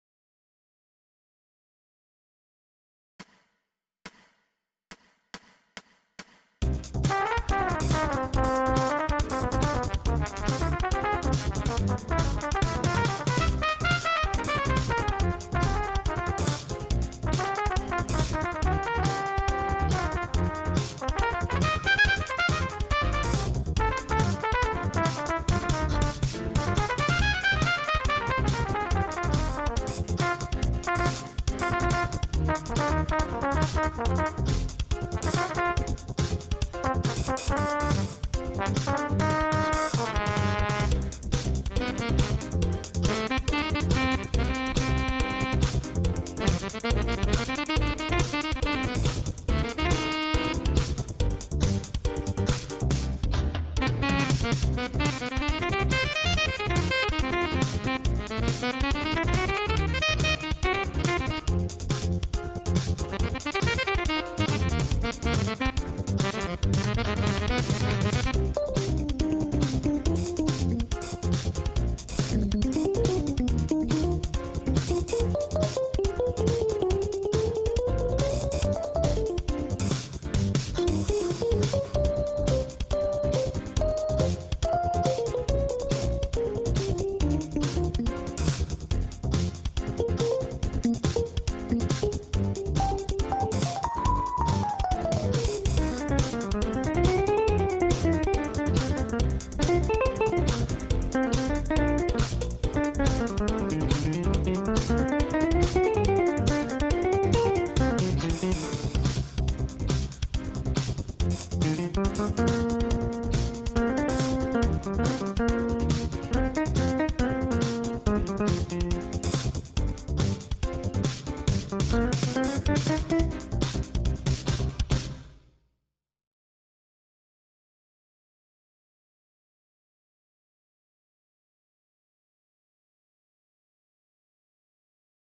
️Use These Funky Trumpet, ePiano, and Guitar Scales to Elevate Your Groove. - Melodic Millennium
"Explore a world of musical possibilities with these funky trumpet, ePiano, and guitar scales. Elevate your groove and take your music to new heights as you dive into the rhythms and melodies of these unique scales. Whether you're a seasoned musician or just starting your musical journey, these scales will add a fresh, vibrant dimension to your playing. Join us for an exciting musical adventure and unlock your creative potential. Let's groove together."